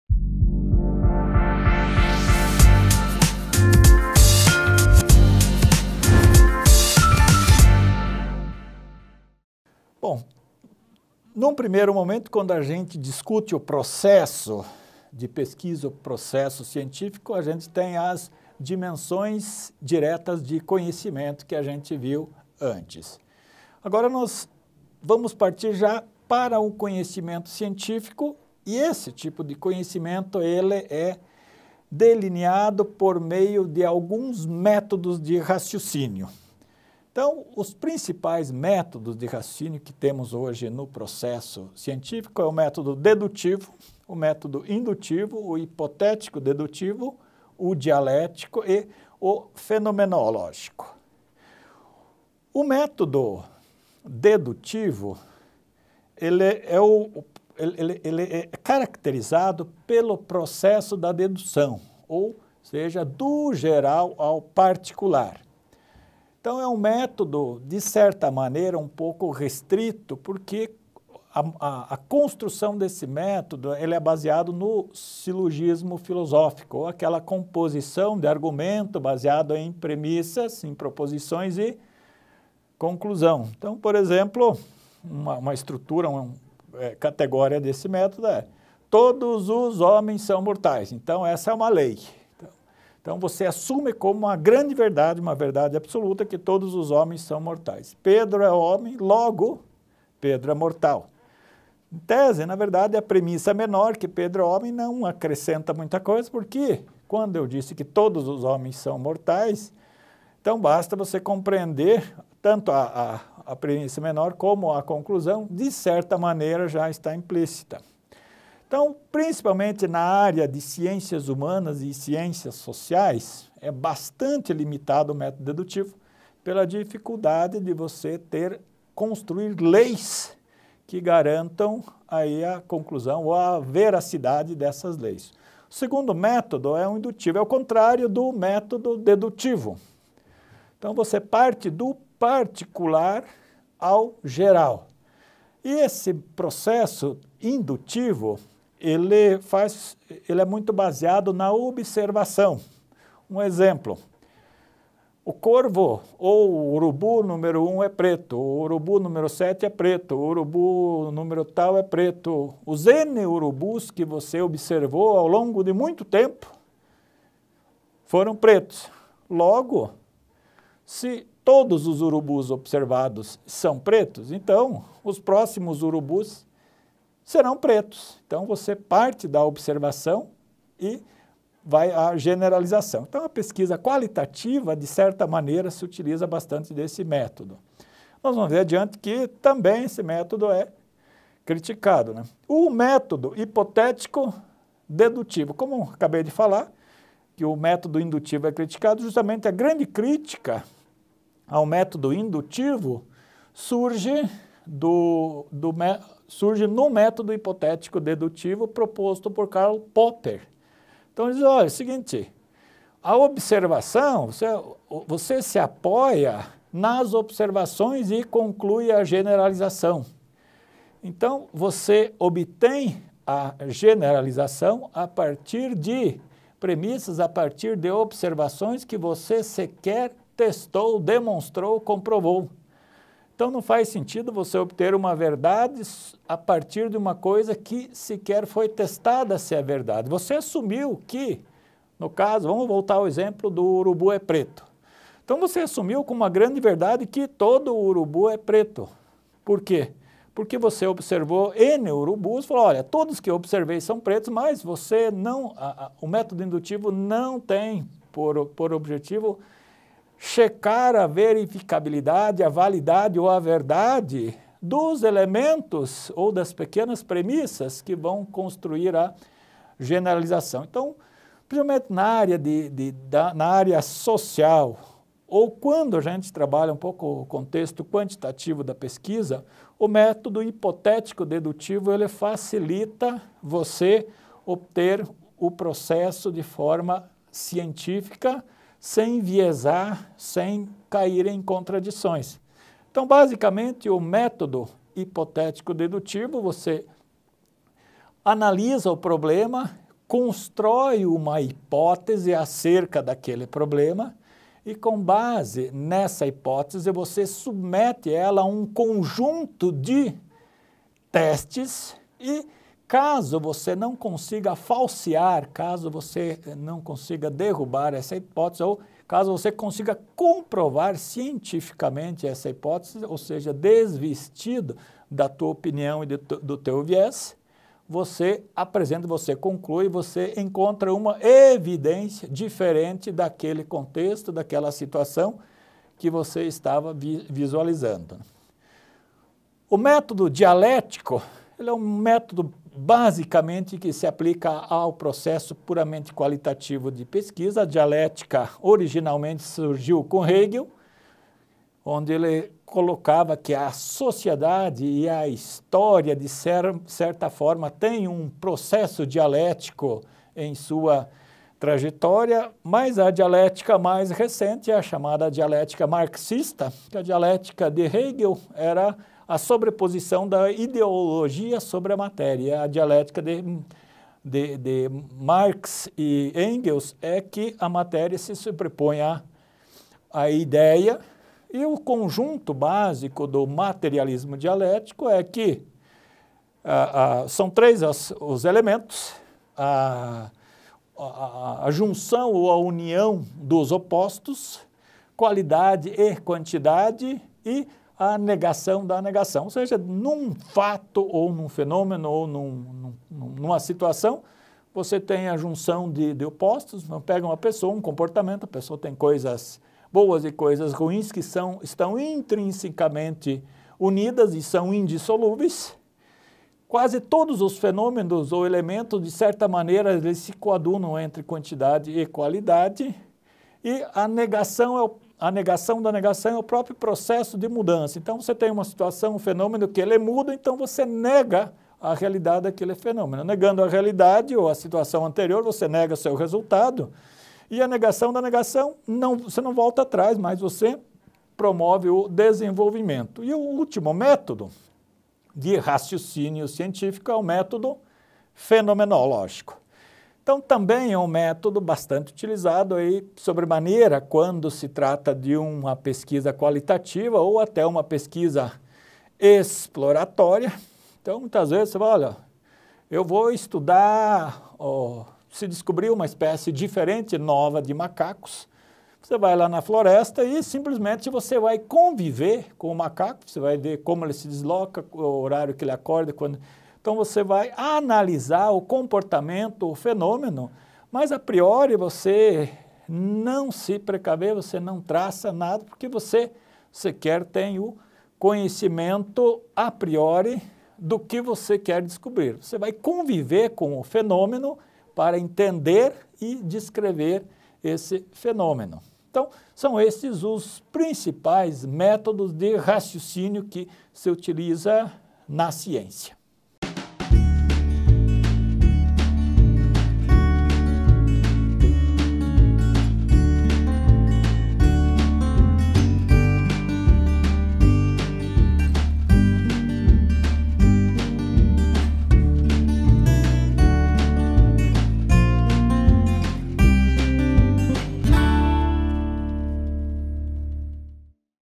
[UVPR] Tipos de Conhecimento
Métodos e Técnicas de Pesquisa - Tipos de Conhecimento com o professor Léo Raifur para o curso de Graduação em Tecnologia em Gestão Pública.